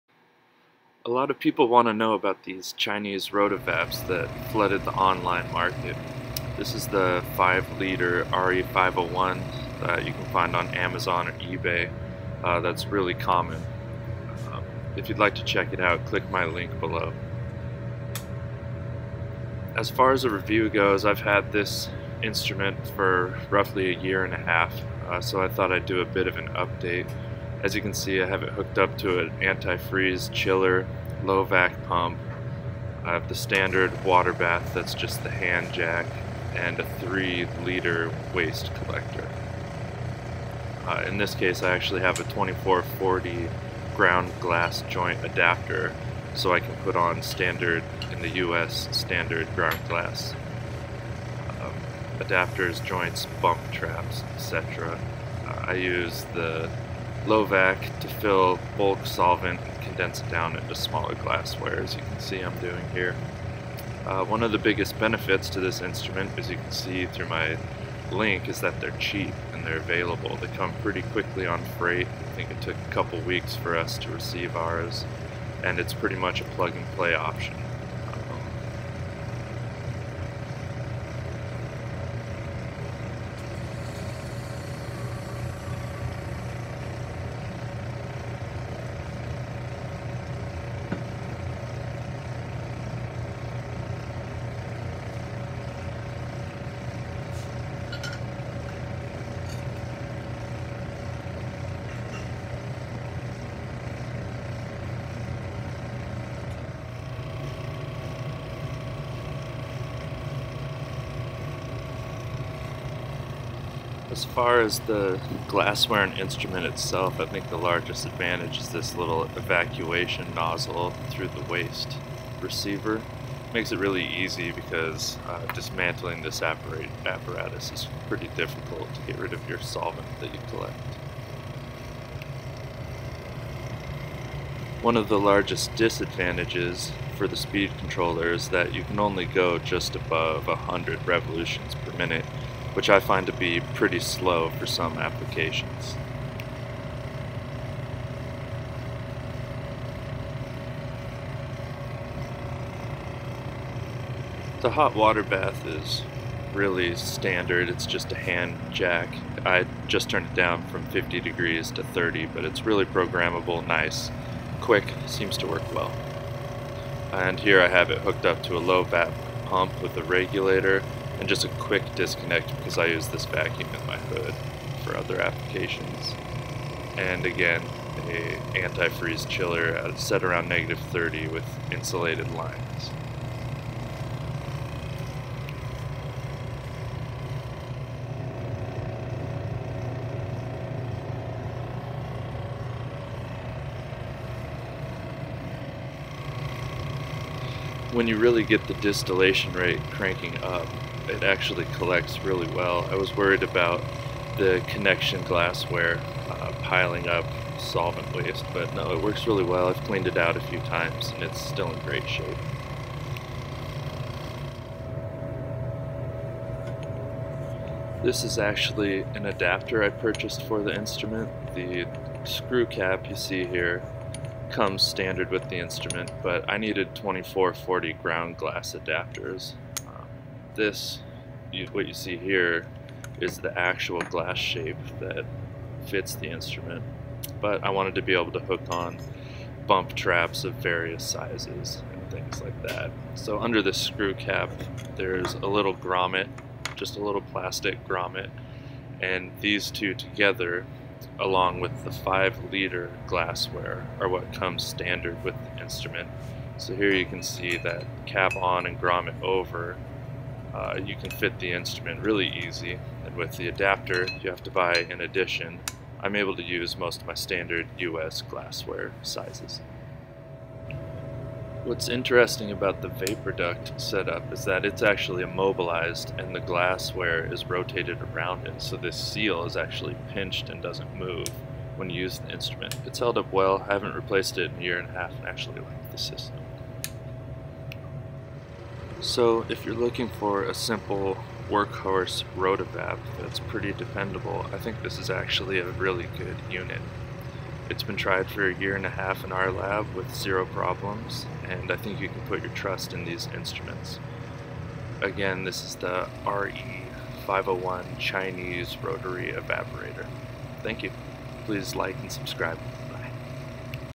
Cheap Chinese Rotavap Review: RE-501 Rotary Evaporator
After 1.5 years of use, I thought it would be interesting to review the RE-501 Chinese rotary evaporator (rotavap) that we purchased. This model is super cheap and we had our reservations upon purchasing the instrument, but everything has worked out pretty well for us and I thought I would share my experience. It is used virtually every work day. Thanks!